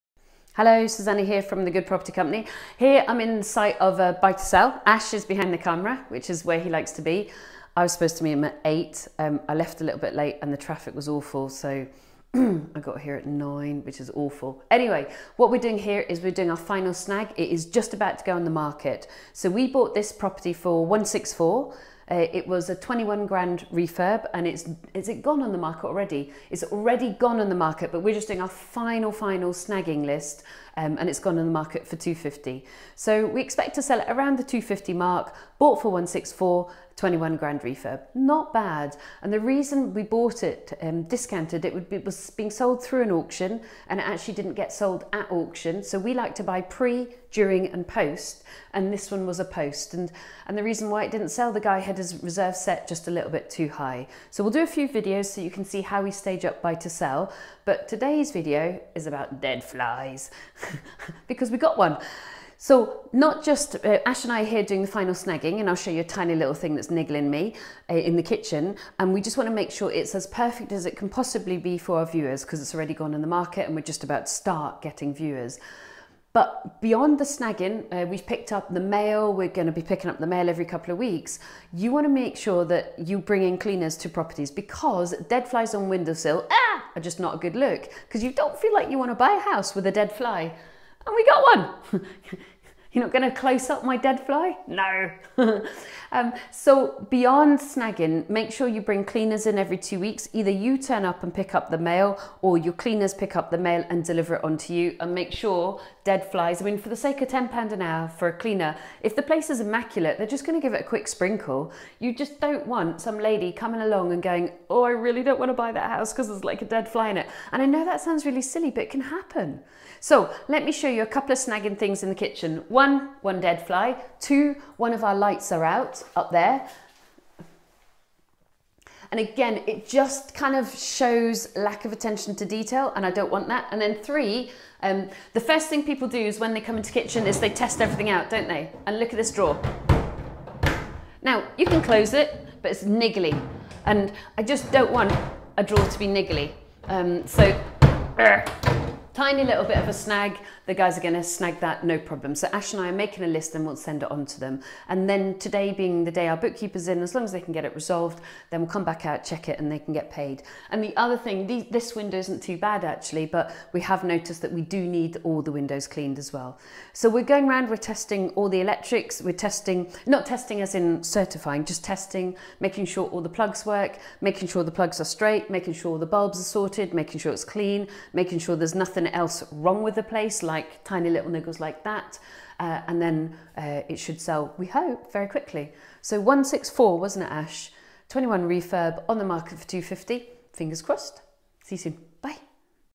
Buy to Sell Property Snagging. Dead Flies not a SEXY LOOK
Susannah Cole shows the final stages of preparing a house for sale.
** This house did not get the offer we wanted, which is always going to be part of the risk of property. So always do PESTLE - Political, Economic, Social, Technological, Legal and Environmental - and look at what might be a problem. For this property, we had BREXIT, and during that time, buyers stepped back largely from buying and so the property did not get the sale. All part of the ups and downs in property -= who on earth would have thought that Brexit would happen, and that was an unexpected economic and political outcome that impacted on house sales during that period.
All part of what you need to look out for.
Good luck all of you.